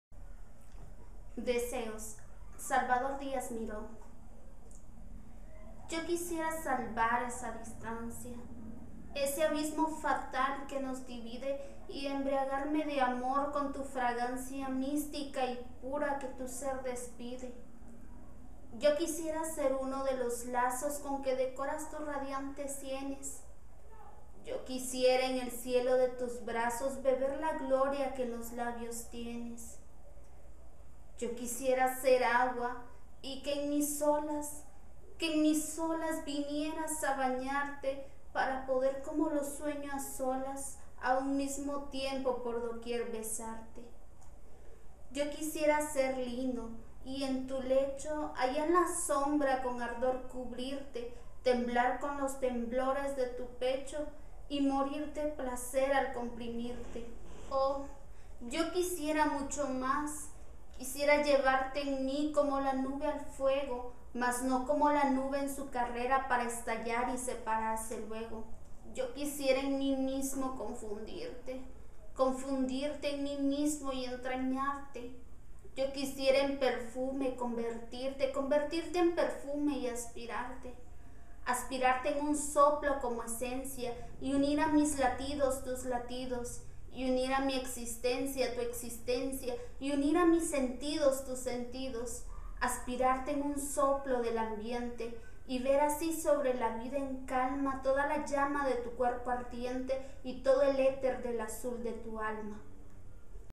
POESÍA | Deseos / Salvador Díaz Mirón
Les dejo un nuevo video, en donde leo un poco del poeta mexicano Salvador Díaz Mirón.